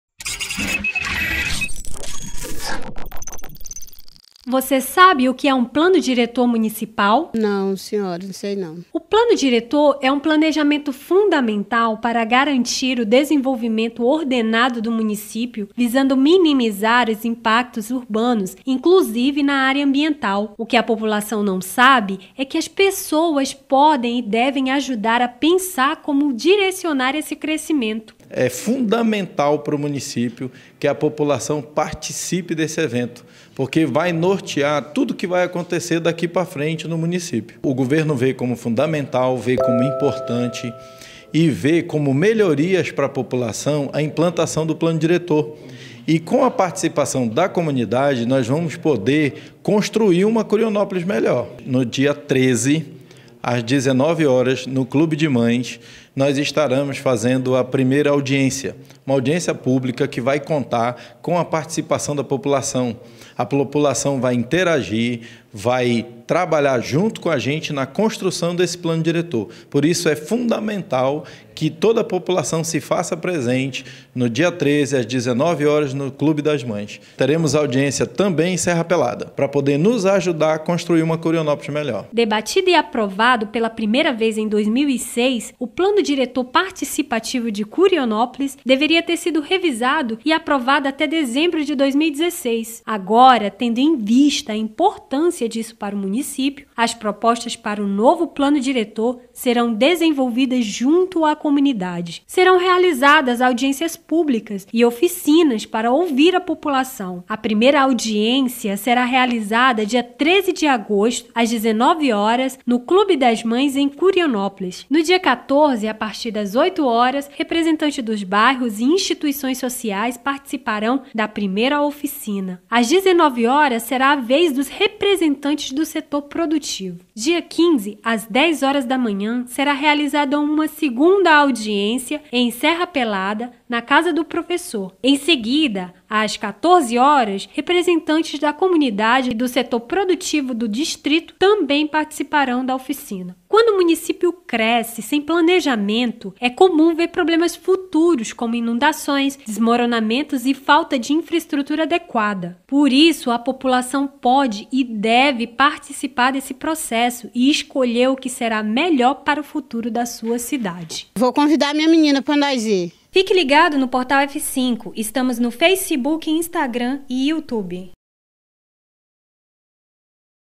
Curionópolis convida a população para audiências de revisão do Plano Diretor
O processo de revisão do Plano Diretor de Curionópolis, conjunto de diretrizes que determina o planejamento de crescimento, ordenamento e o funcionamento da cidade, terá início nesta terça-feira, 13, com a realização de audiências públicas e oficinas para ouvir a comunidade e seus representantes.